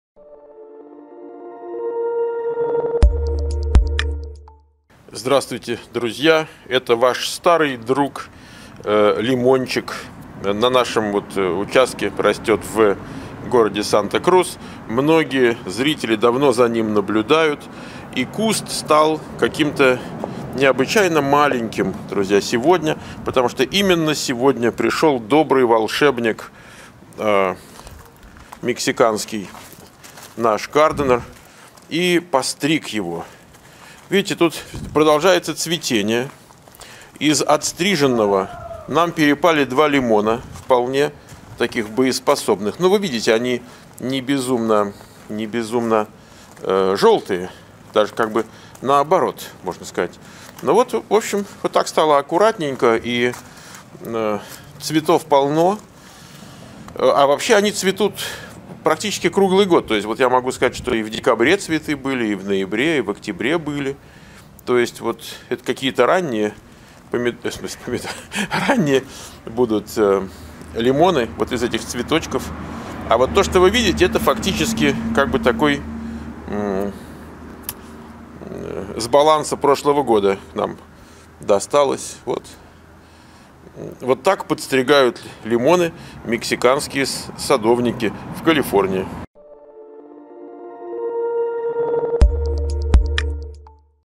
США 4687: Вот так обрезают лимоны мексиканские садовники в Калифорнии
Вот так обрезают лимоны мексиканские садовники в Калифорнии
Ссылки на канале SiliconValleyVoice /школа Михаил Портнов: